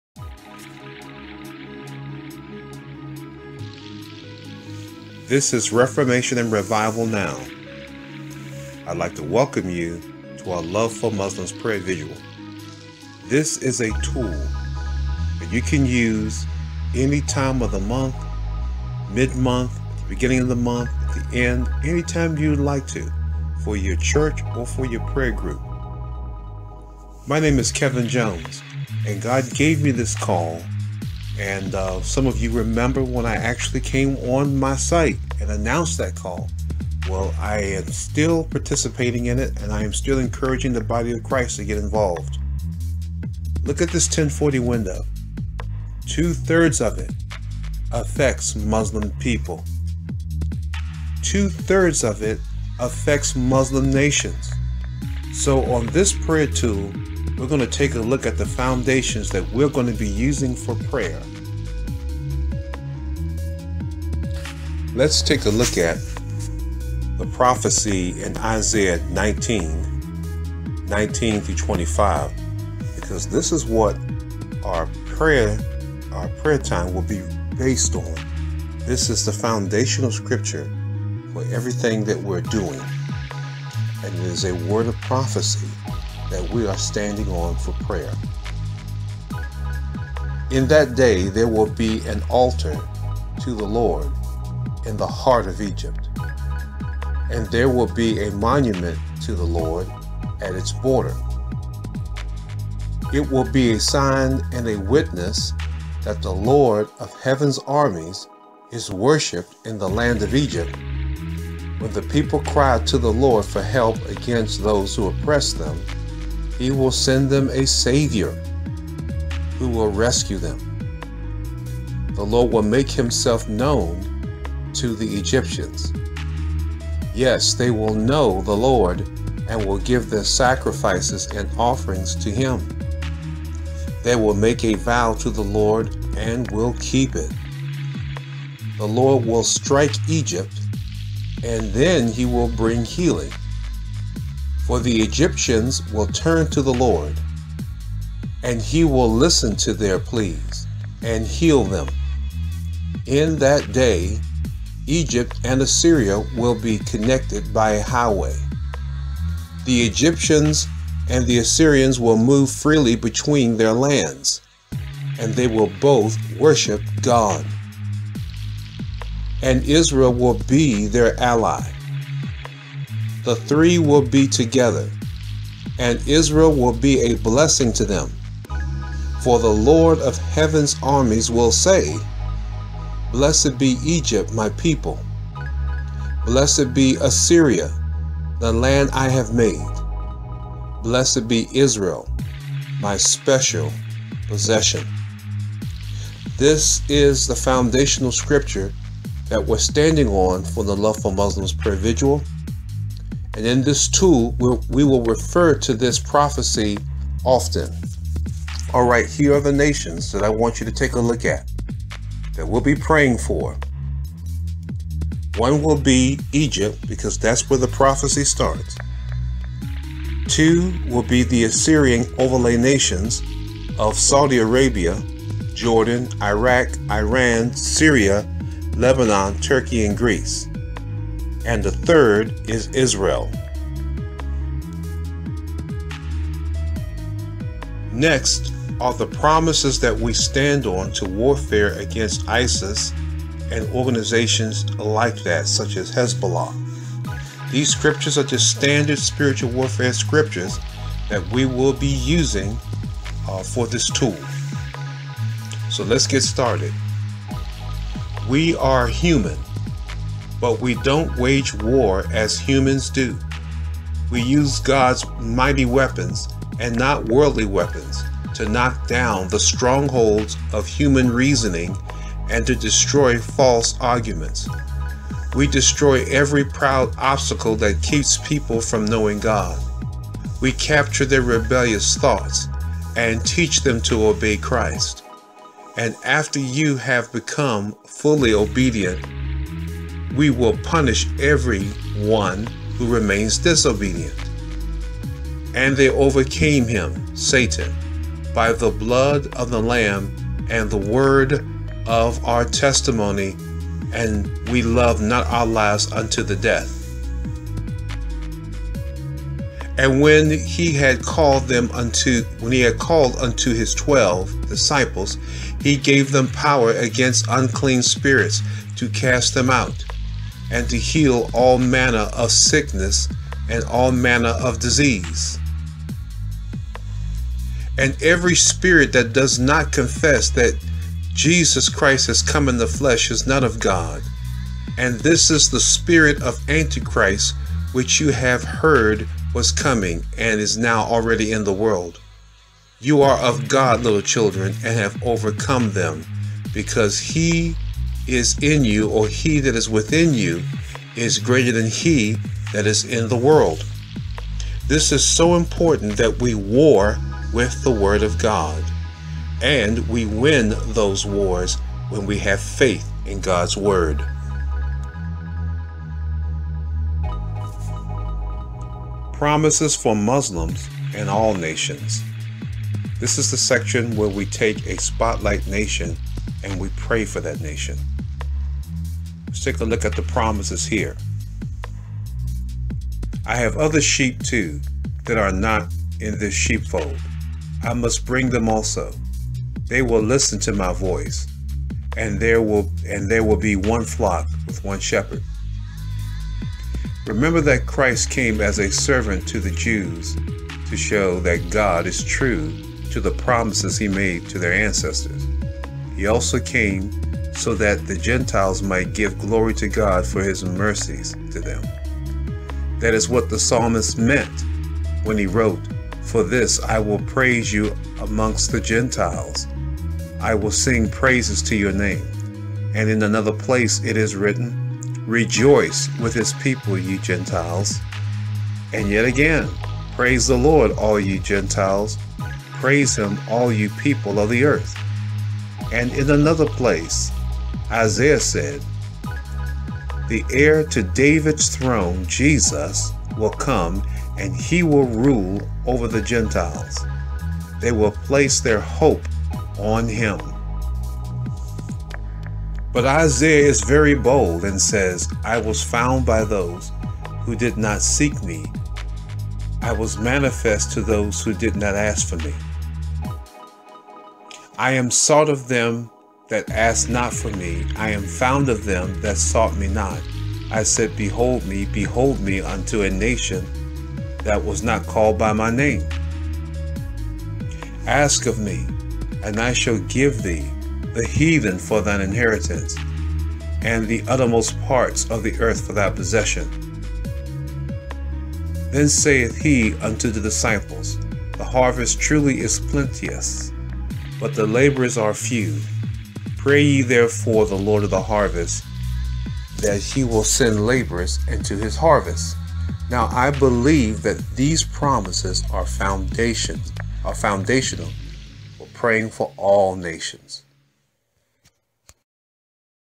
Love for Muslims Prayer Tool intro
We will be participating in the "Love for Muslims Prayer Vigil" (LFMPV) for the month of February, but we will not be going live. Sultana and I are so busy at the moment, but we believe help is on the way soon. We trusting God that"The Lord's Prayer Network" (LPN) will help many in cooperate prayer.